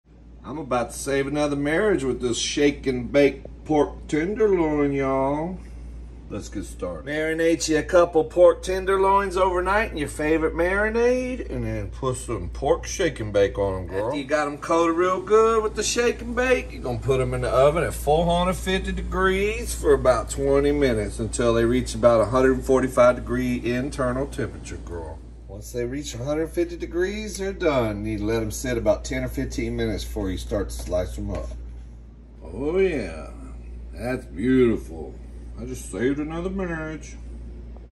SHAKE AND BAKE PORK TENDERLOIN 😂
I bake a Shake and Bake pork tenderloin. cookingchannel food howto cooking howtocook #